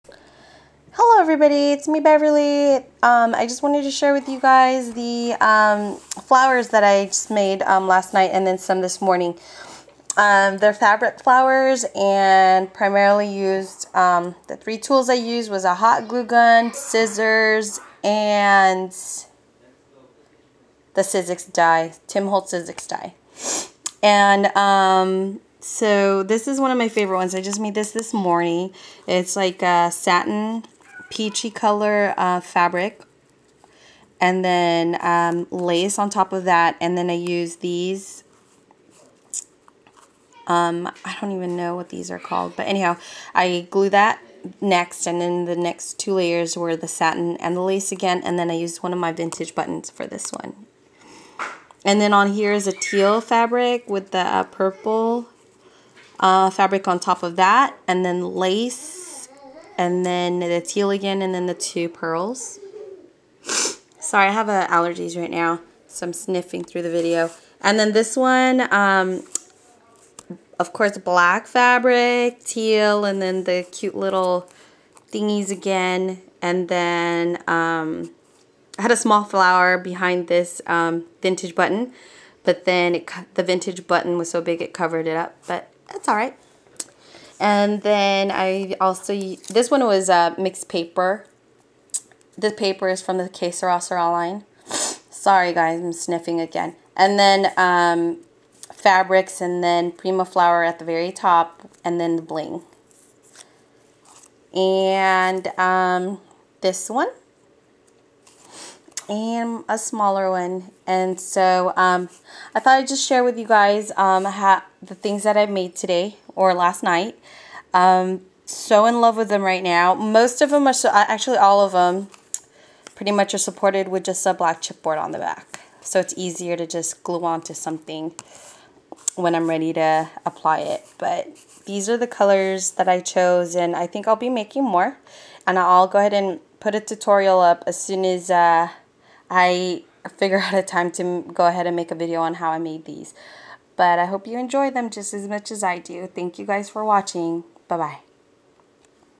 The Piece by Piece: Handmade Fabric Flowers
I used different types of fabrics and lace to make these cute flowers using the Tim Holtz Flower Dies.  I hope you like them.  Enjoy!